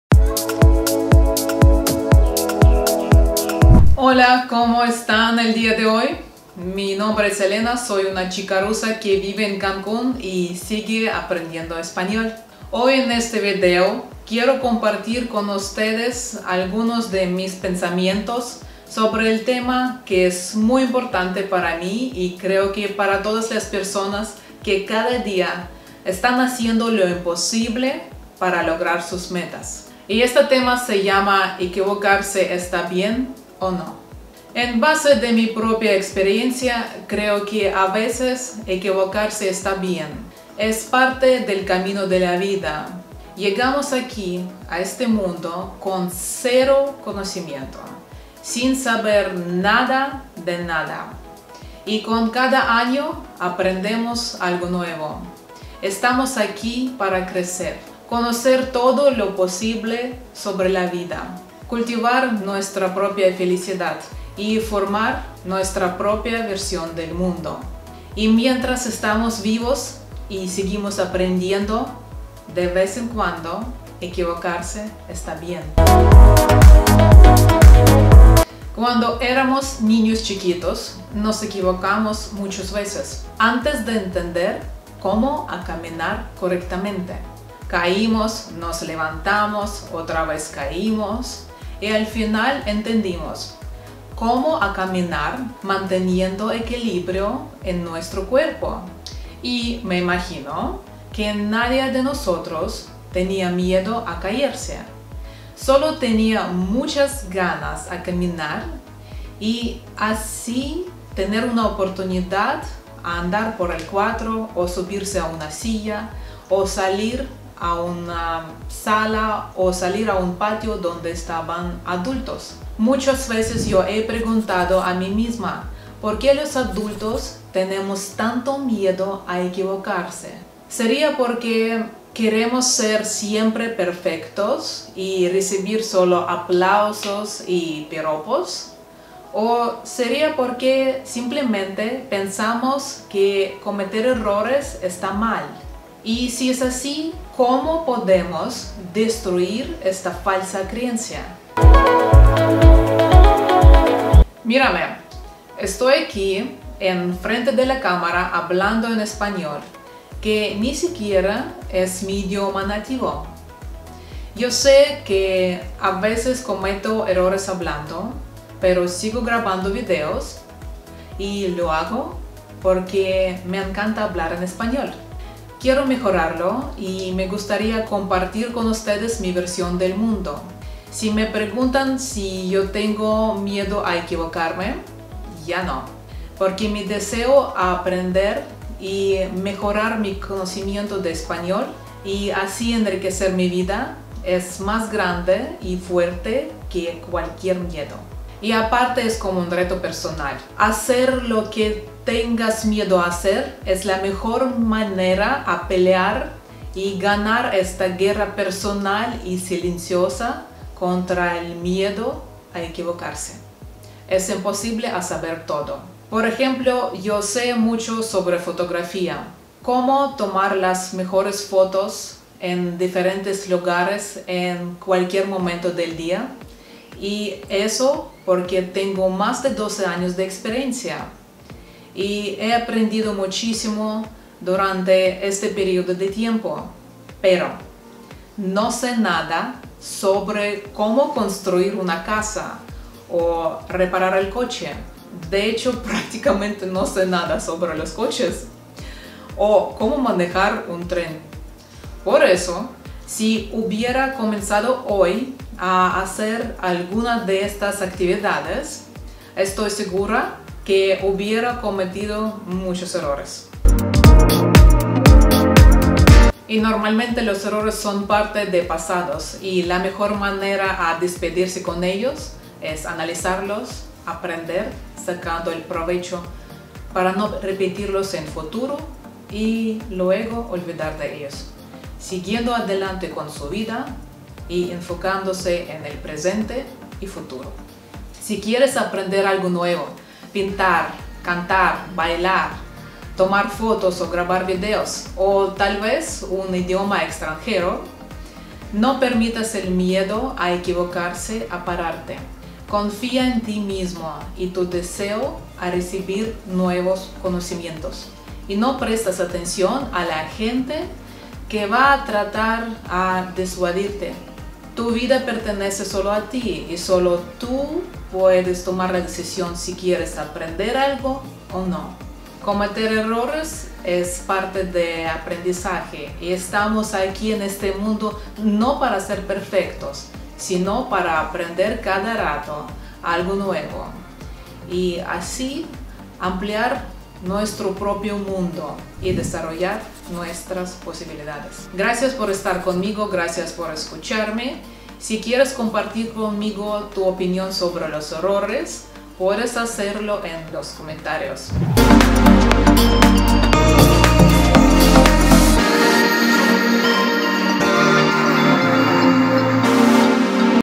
Cómo no equivocarse? Es posible si no tengas miedo a cometer error. No soy perfecta y me equivoqué
Cómo no equivocarse? Y si esto está permitido?

Es posible si no tengas miedo a cometer error. No soy perfecta y me equivoqo, pero lo más importante aprender de los errores, dejarlos en el pasado y seguir adelante con su maravillosa vida!

Extranjeros en Mexico 2020 hablando idioma español. Rusas en Mexico.